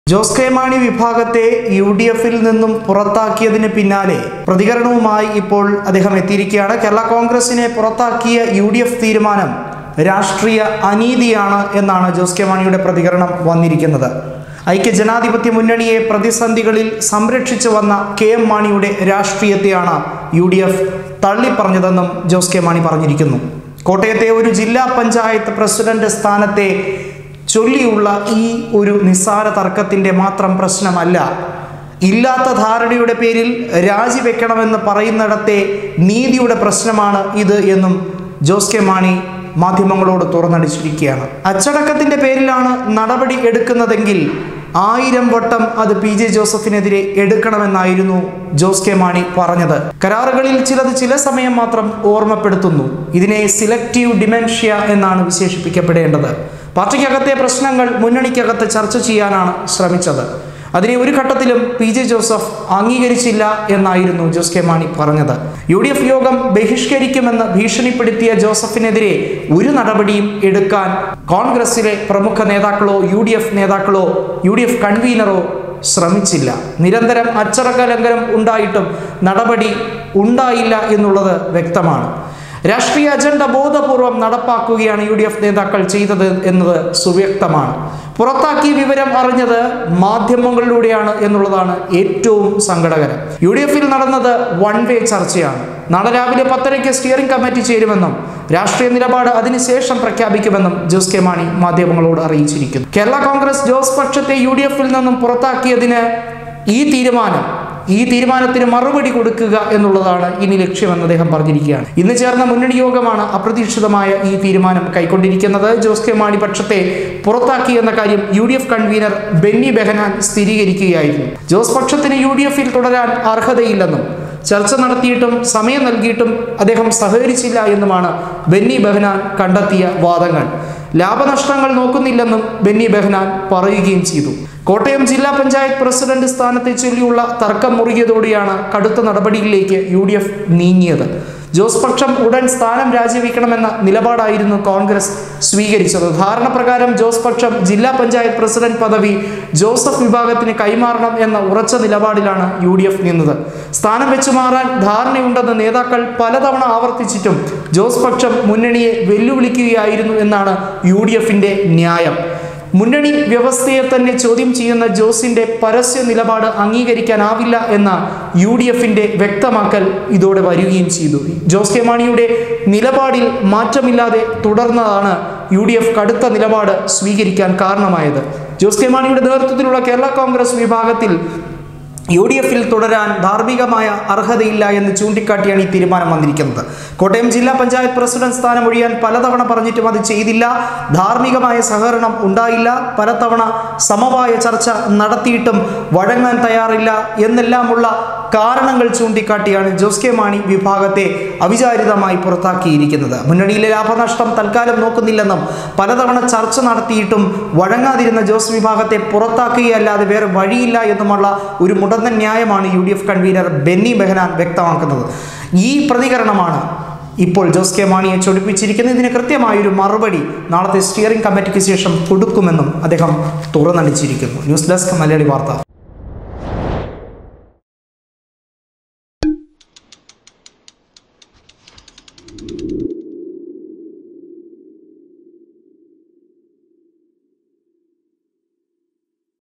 JOSE K MANI പുറത്താക്കിയ വിവരം അറിഞ്ഞത് മാധ്യമങ്ങളിലൂടെ മാത്രം
യുഡിഎഫിൽ നിന്നും പുറത്താക്കിയതിന് പിന്നാലെ പരാതികരണവുമായി : ജോസ് കെ മാണി. കേരളാ കോൺഗ്രസിനെ പുറത്താക്കിയ യുഡിഎഫ് തീരുമാനം രാഷ്ട്രീയ അനീതിയെന്ന് ജോസ് കെ മാണിയുടെ പ്രതികരണം . ഐക്യ ജനാധിപത്യ മുന്നണിയെ പ്രതിസന്ധികളിൽ സംരക്ഷിച്ച് വന്ന കെഎം മാണിയുടെ രാഷ്ട്രീയത്തെയാണ് യുഡിഎഫ് തള്ളിപ്പറഞ്ഞതെന്നും ജോസ് കെ മാണി പറഞ്ഞു. കോട്ടയത്തെ ഒരു ജില്ലാ പഞ്ചായത്ത് പ്രസിഡന്‍റ് സ്ഥാനത്തെ ചൊല്ലിയുള്ള തര്‍ക്കത്തിന്‍റെ മാത്രം പ്രശ്നം അല്ല. ഇല്ലാത്ത ധാരണയുടെ പേരിൽ രാജി വക്കണമെന്ന് പറയുന്നിടത്തെ നീതിയുടെ പ്രശ്നമാണെന്നും ജോസ് കെ മാണി  തുറന്നടിച്ചു.
അച്ചടക്കത്തിന്‍റെ പേരിലാണ് നടപടി എടുത്തതെങ്കിൽ ആയിരം വട്ടം അത് പിജെ ജോസഫിനെതിരെ എടുക്കണമായിരുന്നു എന്നും ജോസ് കെ മാണി. കരാറുകളിൽ ചിലത് ചില സമയത്ത് മാത്രം ഓര്‍മ്മപ്പെടുത്തുന്നു. ഇതിനെ സെലക്ടീവ് ഡിമൻഷ്യ എന്നാണ് വിശേഷിപ്പിക്കേണ്ടത്. പാര്‍ട്ടിക്കകത്തെ പ്രശ്നങ്ങൾ മുന്നണിക്കകത്ത് ചര്‍ച്ച ചെയ്യാനാണ് ശ്രമിച്ചത്.

അതിനെ ഒരു ഘട്ടത്തിലും പിജെ ജോസഫ് അംഗീകരിച്ചിരുന്നില്ലെന്നും ജോസ് കെ മാണി പറഞ്ഞു.  യുഡിഎഫ് യോഗം ബഹിഷ്കരിക്കുമെന്ന് ഭീഷണിപ്പെടുത്തിയ ജോസഫിനെതിരെ നടപടിയില്ല. നിരന്തരം അച്ചടക്കം ലംഘിച്ചിട്ടും നടപടി ഉണ്ടായില്ല. രാഷ്ട്രീയ അജണ്ട ബോധപൂര്‍വ്വം നടപ്പാക്കുകയാണ് യുഡിഎഫ് നേതാക്കൾ ചെയ്തത്. പുറത്താക്കിയ വിവരം അറിഞ്ഞത് മാധ്യമങ്ങളിലൂടെയാണ്. യുഡിഎഫിൽ നടന്നത് വൺവേ ചർച്ചയാണ്.  നാളെ രാവിലെ പത്തരയ്ക്ക് സ്റ്റിയറിംഗ് കമ്മിറ്റി ചേരും . രാഷ്ട്രീയ നിലപാട് അതിന് ശേഷം പ്രഖ്യാപിക്കുമെന്നും ജോസ് കെ മാണി അറിയിച്ചു.

  കേരള കോണ്‍ഗ്രസ്-ജോസ് പക്ഷത്തെ യുഡിഎഫിൽ നിന്നും പുറത്താക്കിയ തീരുമാനം ഇന്ന് എടുക്കുകയായിരുന്നു . ഇന്ന് ചേർന്ന മുന്നണിയോഗമാണ് അപ്രതീക്ഷിത തീരുമാനം കൈക്കൊണ്ടത്. ജോസ് കെ. മാണി പക്ഷത്തെ പുറത്താക്കിയെന്ന കാര്യം യുഡിഎഫ് കണ്‍വീനർ ബെന്നി ബെഹനാൻ സ്ഥിരീകരിച്ചു.. ജോസ് പക്ഷത്തിനു യു ഡി എഫിൽ തുടരാൻ അർഹതയില്ലെന്നും ചർച്ച നടത്തിയിട്ടും സമയം നൽകിയിട്ടും സഹകരിച്ചില്ല.ലാഭനഷ്ടങ്ങൾ നോക്കുന്നില്ലെന്നും ബെന്നി ബെഹനാൻ പറഞ്ഞു ,

കോട്ടയം ജില്ലാ പഞ്ചായത്ത് പ്രസിഡന്‍റ് സ്ഥാനത്തെ ചൊല്ലിയുള്ള തര്‍ക്കം മുറുകിയതോടെയാണ് കടുത്ത നടപടിയിലേക്ക് യു ഡി എഫ് നീങ്ങിയത് . ജോസ് പക്ഷം ഉടൻ സ്ഥാനം രാജിവെക്കണമെന്ന നിലപാടായിരുന്നു കോൺഗ്രസ് സ്വീകരിച്ചത് . ധാരണ പ്രകാരം ജോസ് പക്ഷം ജില്ലാ പഞ്ചായത്ത് പ്രസിഡന്‍റ് പദവി ജോസഫ് വിഭാഗത്തിന് കൈമാറണം എന്ന ഉറച്ച നിലപാടിലാണ് യുഡിഎഫ്. സ്ഥാനം വെച്ചുമാറാൻ ധാരണ ഉണ്ടെന്ന് നേതാക്കൾ പല തവണ ആവർത്തിച്ചിട്ടും ജോസ് പക്ഷം മുന്നണിയെ വെല്ലുവിളിക്കുന്നെന്നാണ് യുഡിഎഫ് നിലപാട്. മുന്നണി വ്യവസ്ഥയെ തന്നെ ചോദ്യം ചെയ്യുന്ന ജോസിന്‍റെ പരസ്യ നിലപാട് അംഗീകരിക്കാനാകില്ലെന്ന് യുഡിഎഫ് വ്യക്തമാക്കിയിട്ടുണ്ട്.

ജോസ് കെ മാണിയുടെ നിലപാടിൽ മാറ്റമില്ലാത്തതിനെ തുടർന്നാണ് യു ഡി എഫ് കടുത്ത നിലപാട് സ്വീകരിച്ചത് ജോസ് കെ മാണിയുടെ നേതൃത്വത്തിലുള്ള കേരളാ കോൺഗ്രസ് വിഭാഗത്തിന് യുഡിഎഫിൽ തുടരാൻ ധാർമികമായ അർഹതയില്ലെന്ന് ചൂണ്ടിക്കാട്ടിയാണ് തീരുമാനം. കോട്ടയം ജില്ലാ പഞ്ചായത്ത് പ്രസിഡന്‍റ് സ്ഥാനം ഒഴിയാൻ പല തവണ പറഞ്ഞിട്ടും അത് ചെയ്തില്ല, ധാർമികമായ സഹകരണം ഉണ്ടായില്ല, പല തവണ സമവായചർച്ച നടത്തിയിട്ടും വഴങ്ങാൻ തയ്യാറായില്ല എന്നെല്ലാമുള്ള കാരണങ്ങൾ ചൂണ്ടിക്കാട്ടിയാണ് ജോസ് കെ മാണി വിഭാഗത്തെ പുറത്താക്കിയത്. മുന്നണിയിലെ ലാഭനഷ്ടം തൽക്കാലം നോക്കുന്നില്ലെന്നും പല തവണ ചർച്ച നടത്തിയിട്ടും വഴങ്ങാതിരുന്ന ജോസ് വിഭാഗത്തെ പുറത്താക്കുകയല്ലാതെ വേറെ വഴിയില്ലെന്ന് യുഡിഎഫ് കൺവീനർ ബെന്നി ബഹനാൻ വ്യക്തമാക്കുന്നു.
ഇതിനോട് പ്രതികരിക്കുകയായിരുന്നു.